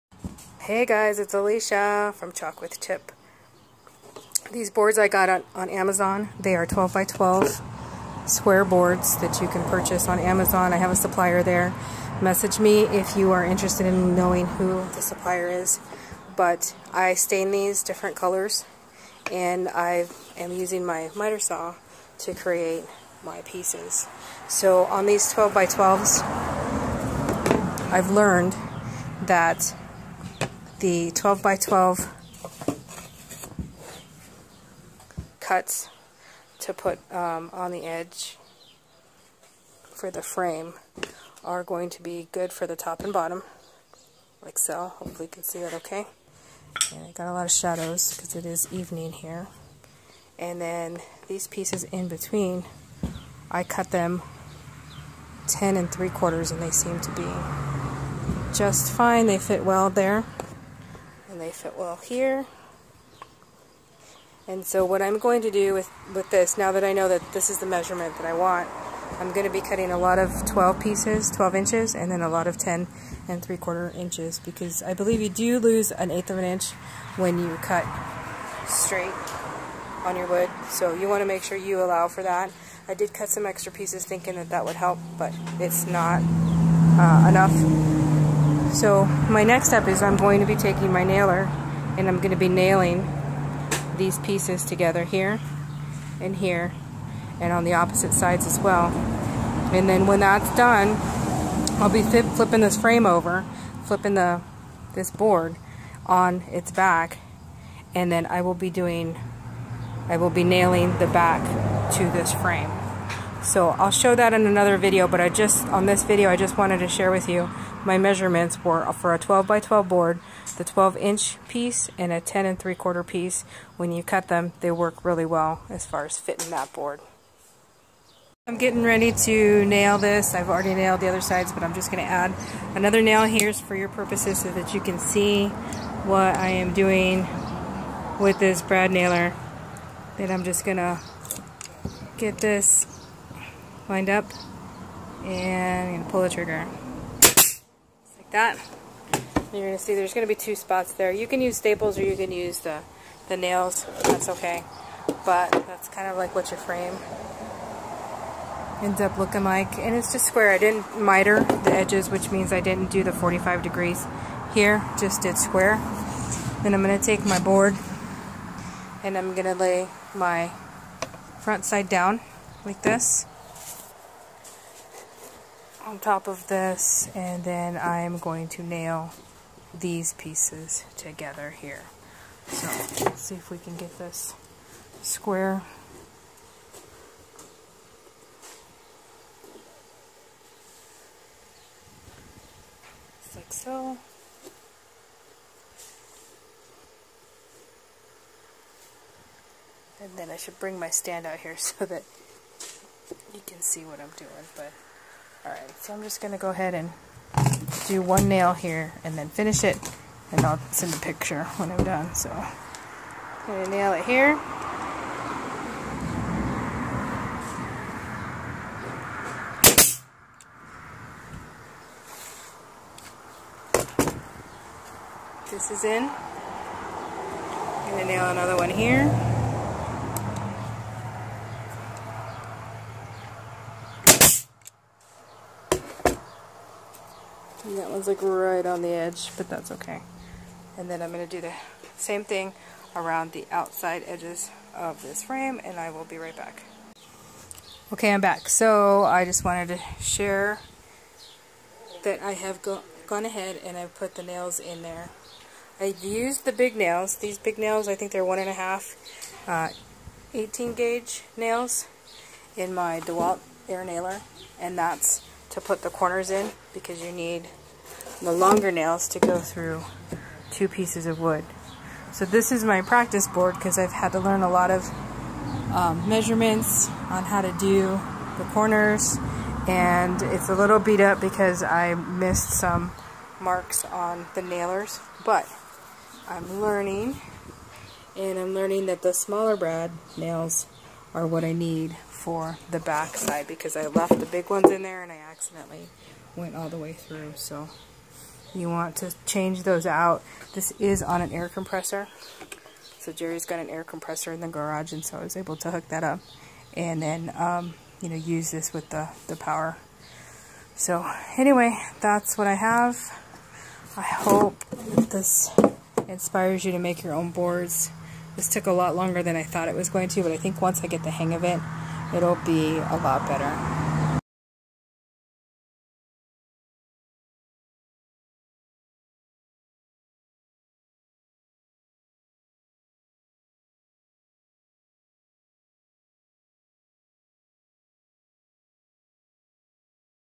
Chalk with Chip DIY Farmhouse Box Frame
Watch Chip DIY her first box frame with a few power tools.